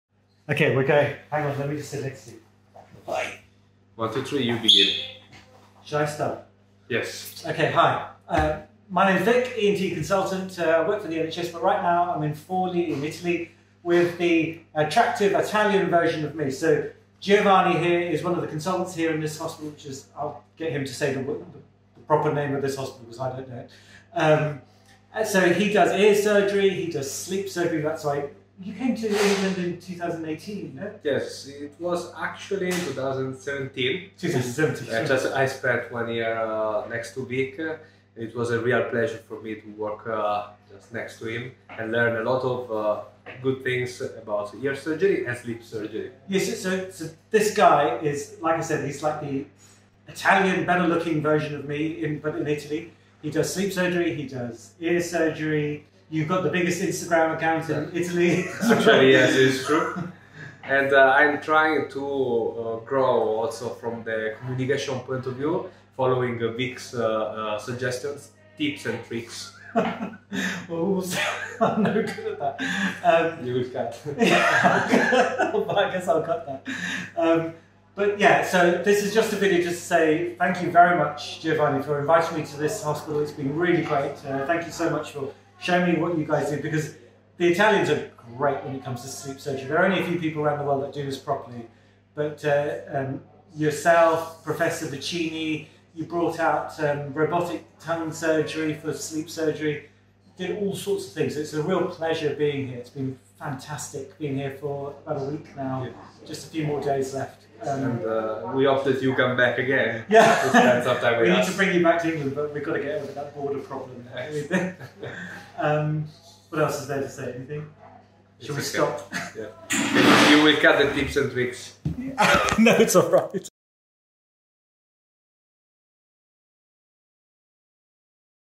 Surgery for Snoring & Sleep Apnoea in Italy
I often travel to meet other experts in surgery for snoring & sleep apnoea around the world to make sure I am giving my patients the very best care.  I have just come back from a week working in Forli Italy meeting an old friend of mine (Giovanni Cammaroto). Giovanni is an ENT Consultant surgeon (attending), who has the largest ENT Instagram account in Italy. I want to thank him for showing me around for a week in his hospital.

Instagram handle is otorinolaringoiatria_2.0

This video also shows the benefit of scripting, editing and at least thinking about what you are going to say before you do.